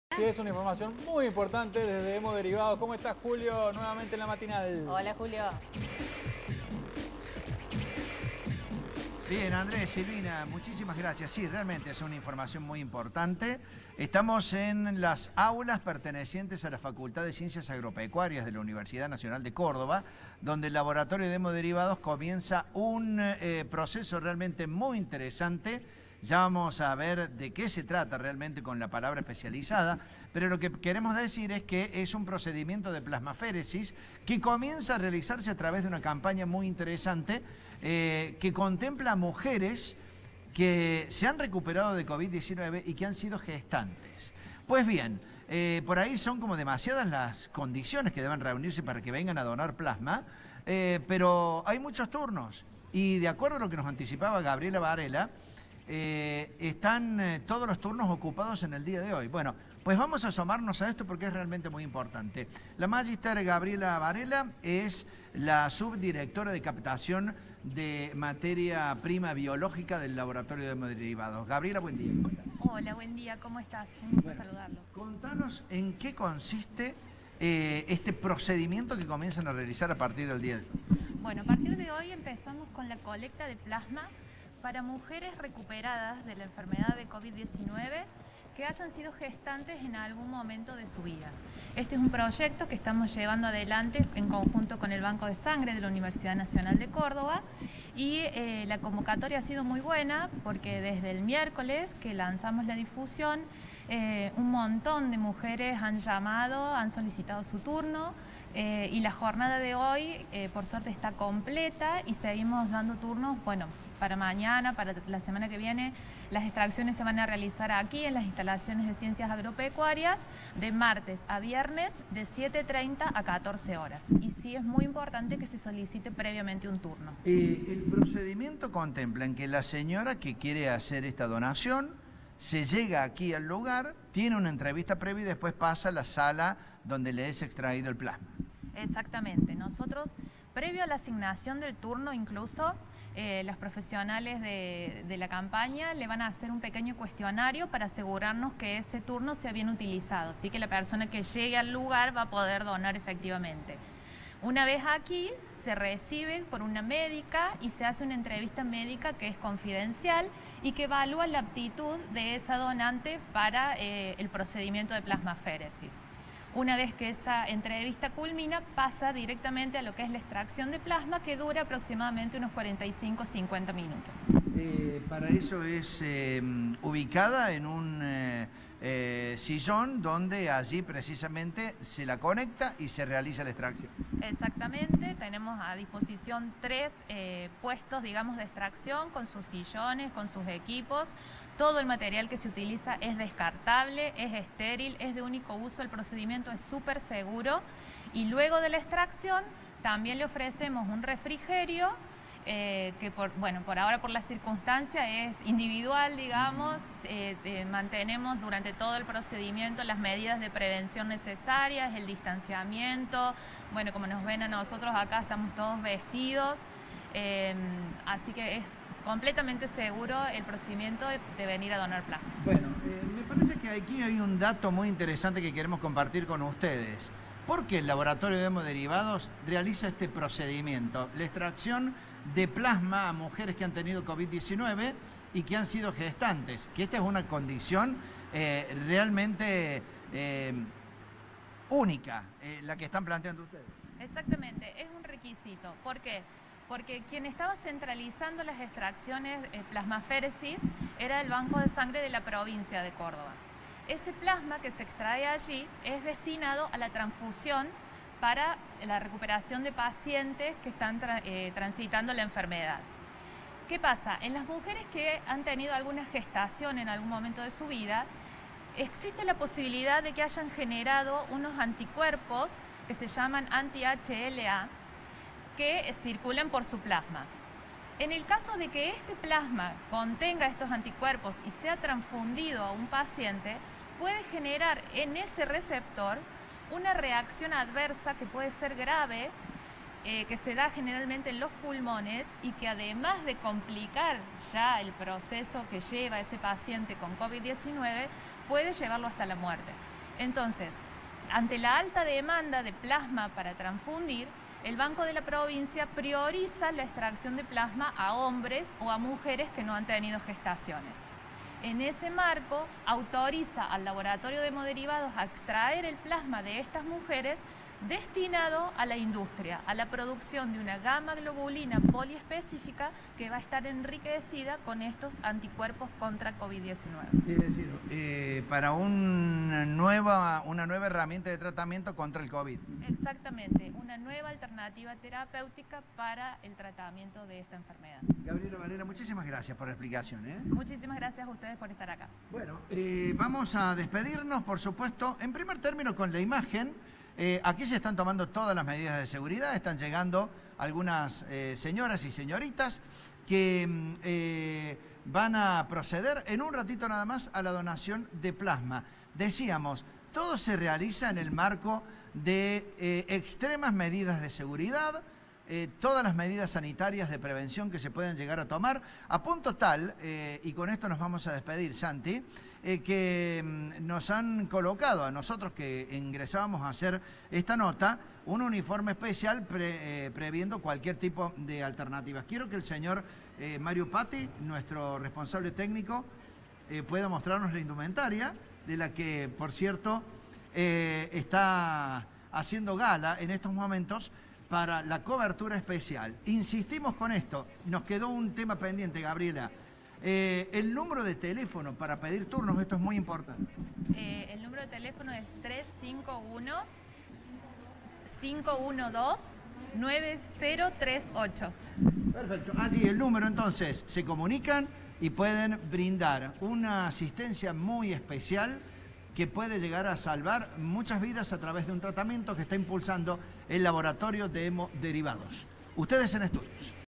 Hemoderivados inicia campaña de plasma para mujeres recuperada
Móvil de Julio Kloppenburg desde Ciudad Universitaria